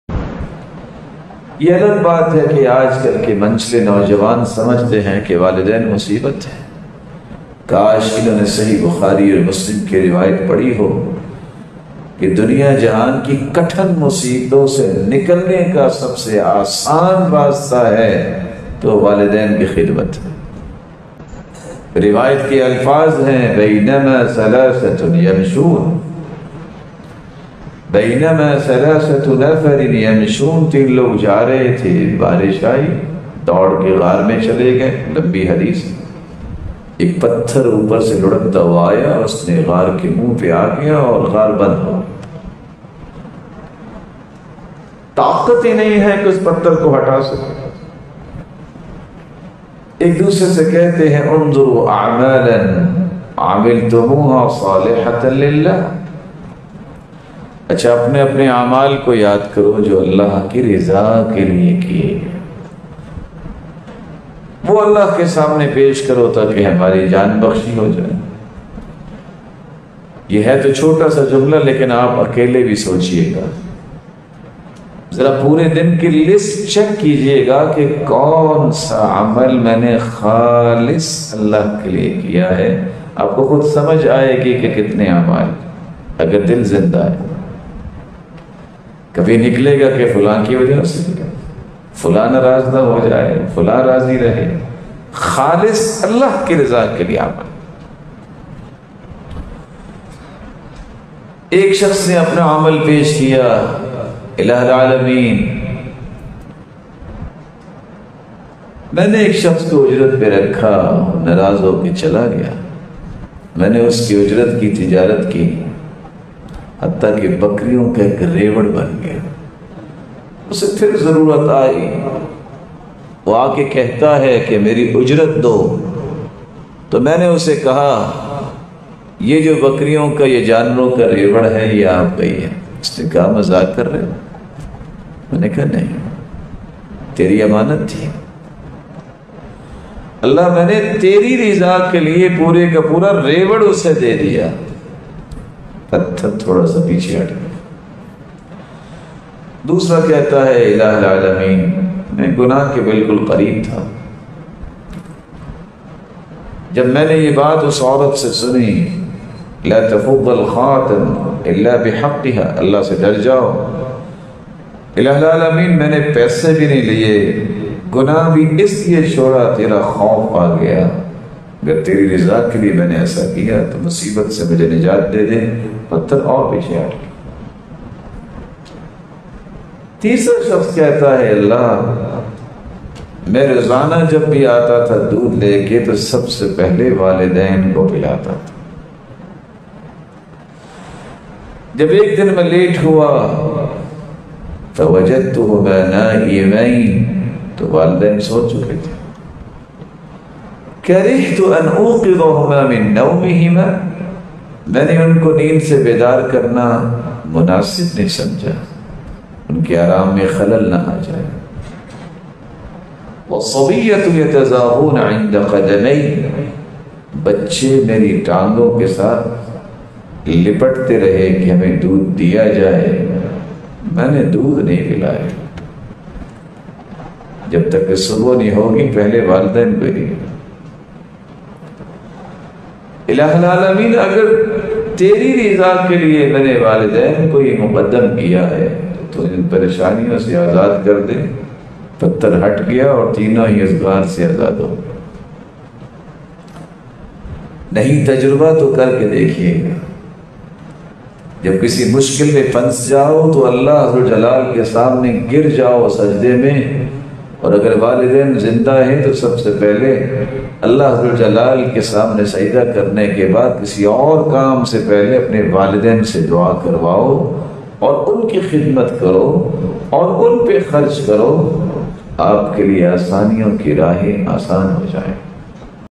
Walidain Ki Khidmat Ki Fazilat | Qari Sohaib Ahmed Meer Muhammadi New Bayan 2022
Speeker : Qari Sohaib Ahmed Meer Muhammadi
Chief Executive : Quraan College and Islamic Training Institutea, Phool Nager
District Kasur, Pakistan.